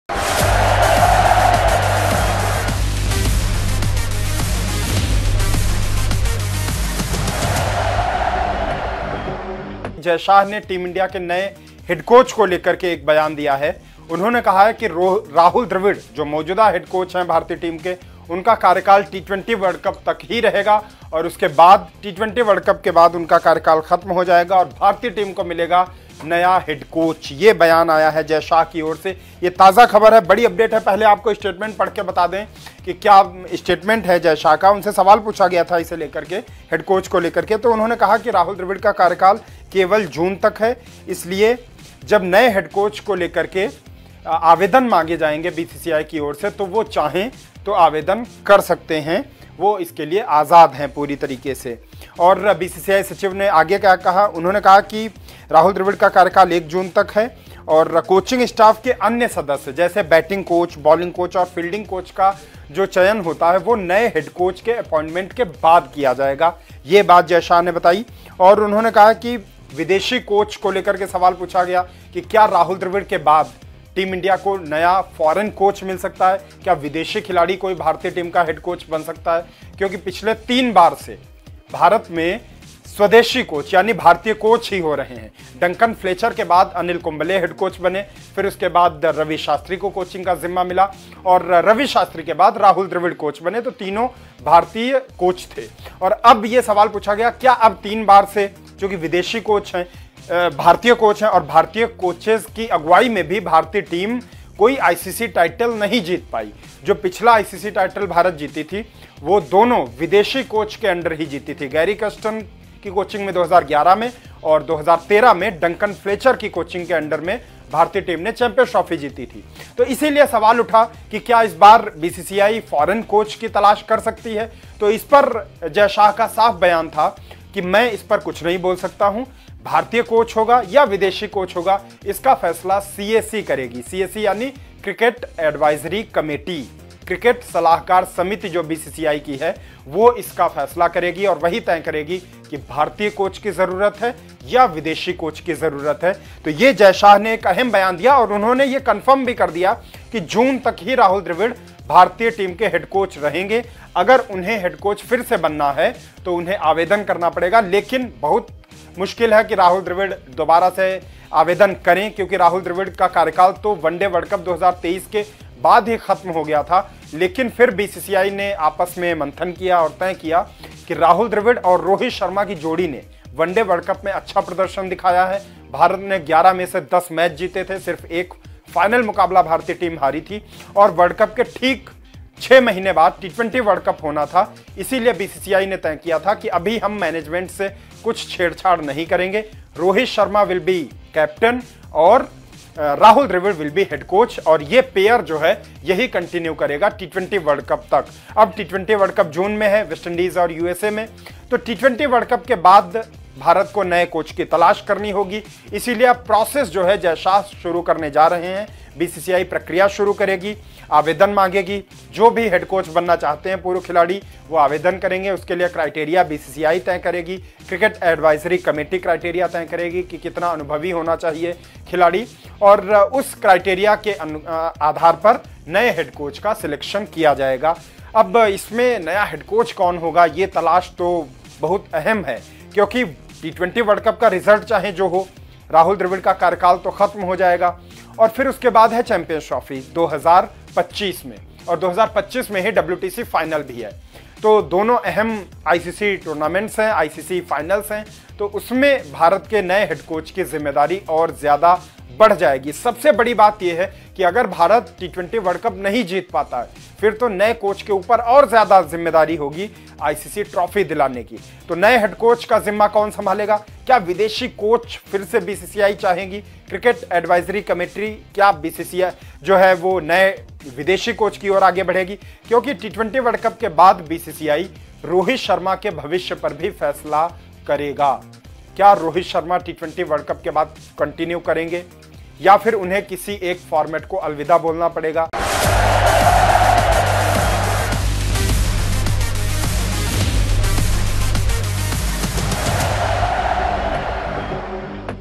New Head Coach in Team India Breaking News: Jay Shah का ऐलान, Rahul Dravid पर BCCI का फरमान
भारतीय टीम को जल्द ही नया मुख्य कोच मिल सकता है. इसका खुलासा खुद BCCI के सचिव Jay Shah ने कर दिया है. हाल ही में सामने आई रिपोर्ट के मुताबिक बोर्ड जल्द ही मुख्य कोच के पद के लिए विज्ञापन जारी करने जा रहा है.